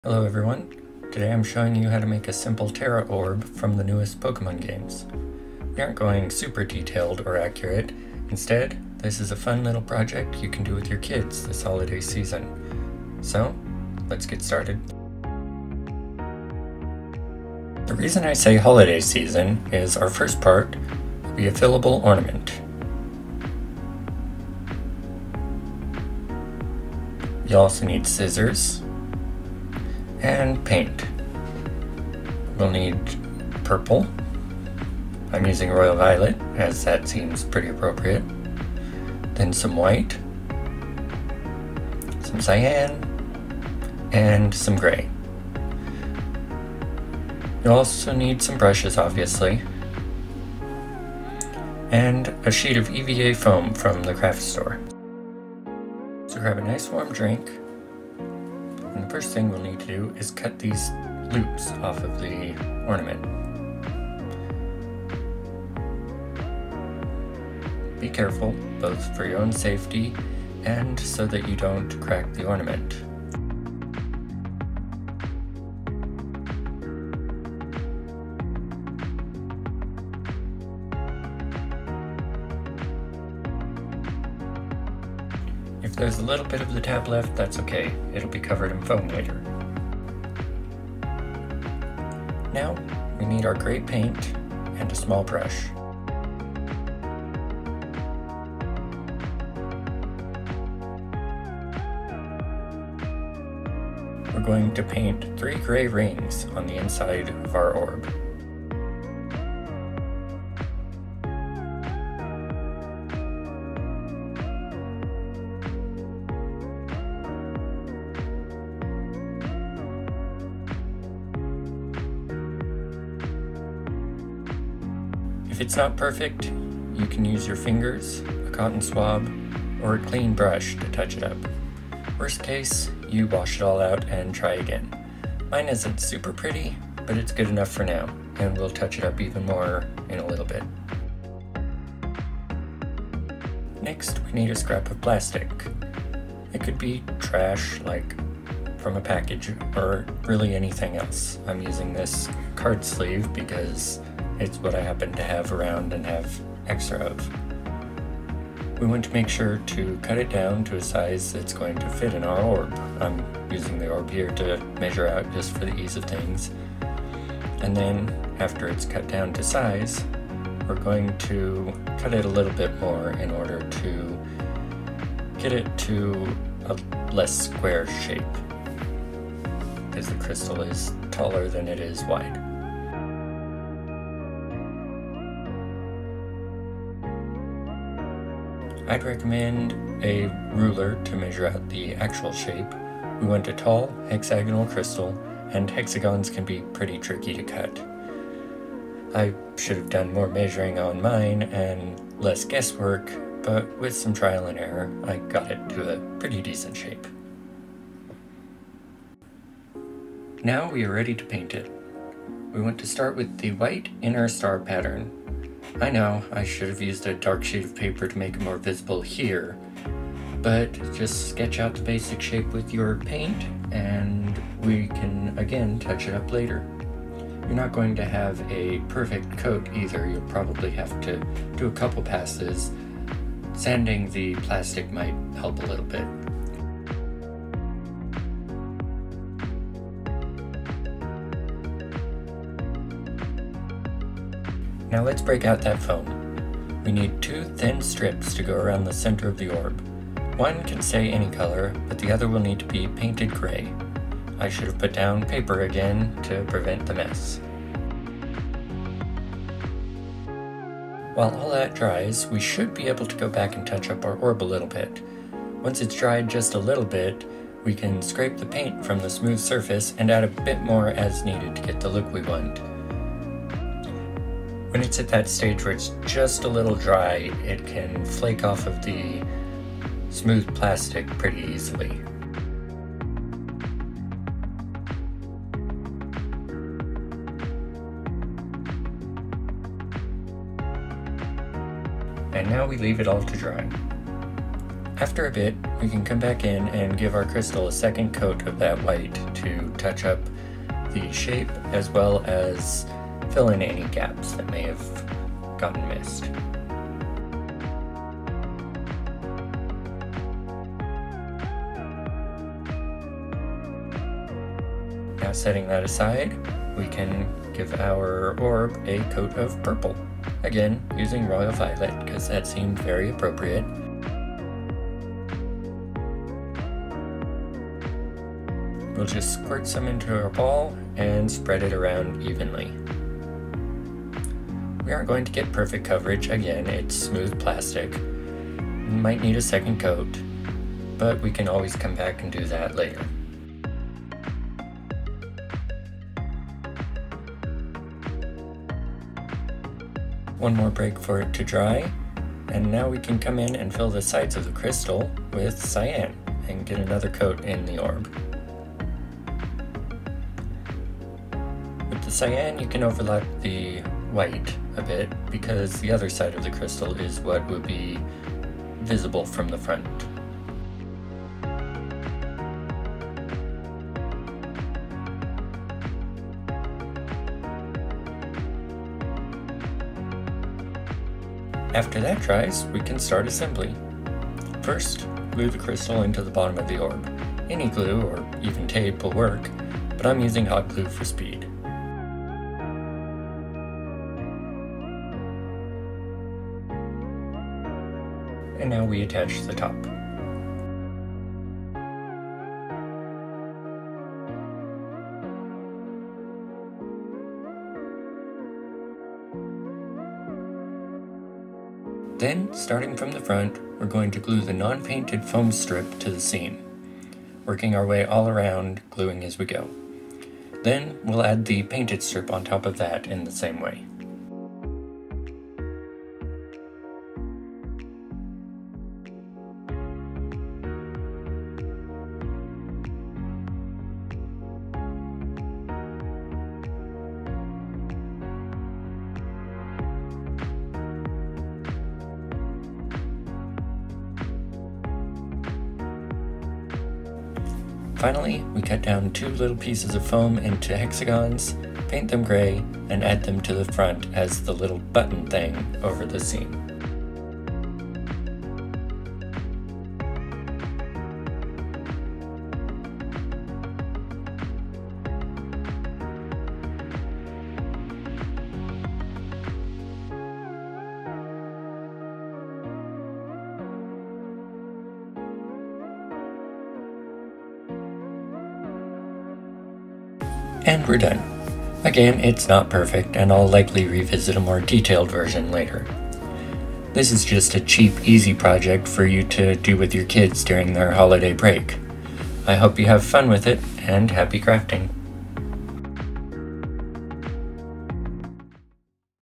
How To Make Your Own Tera Orbs!
Check it out, my first video!

I wanted to make a super simple tera orb and, right now, clear plastic ornaments are easy to find! Everything for this project should be cheap and easy to come by for you to make with your kids.